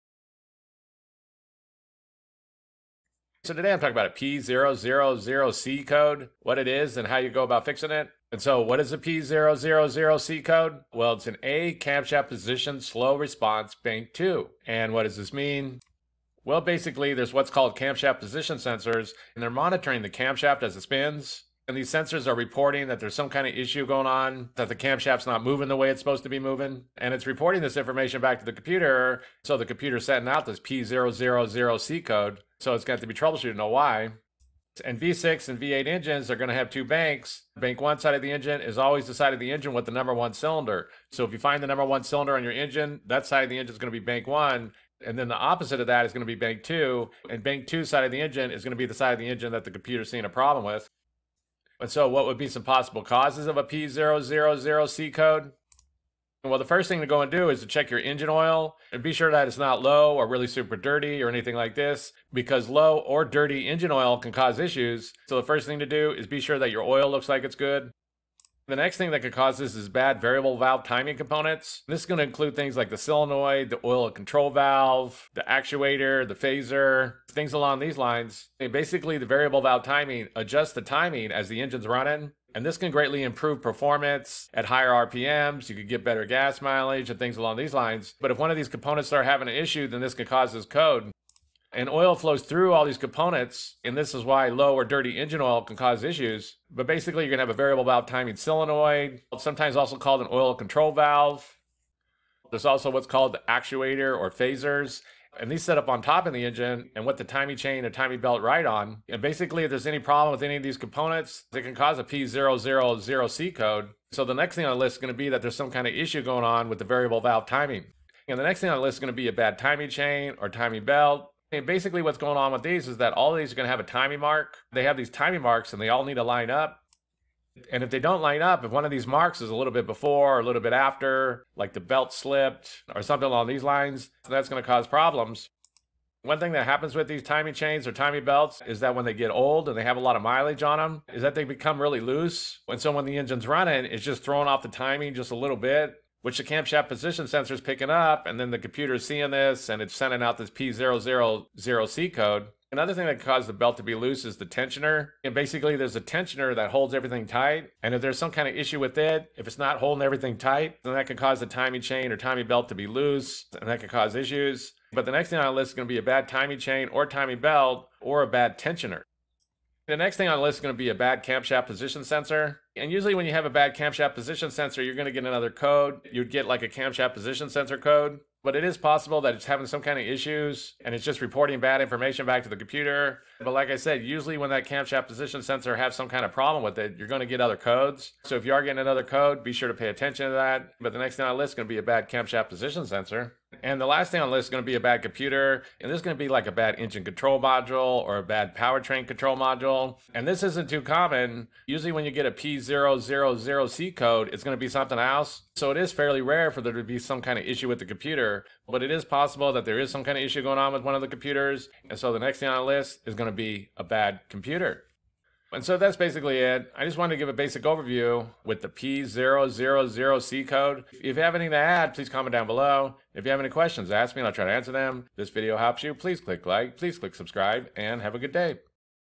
Causes and Fixes P000C Code: A Camshaft Position Slow Response Bank 2
How to fix P000C Code: A Camshaft Position Slow Response Bank 2
A P000C code is set when the computer (ECM/PCM) detects that the camshaft’s position differs from the position that the computer expects during camshaft phase changes.

Possible Causes of a P000C Code:
Low or Dirty Engine Oil
Bad VVT Components (Solenoid, Oil Control Valve, Actuator/Phaser)
Bad Timing Chain or Timing Belt
Bad Timing Chain Tensioner
Bad Cam Position Sensor
Bad Computer (ECM/PCM)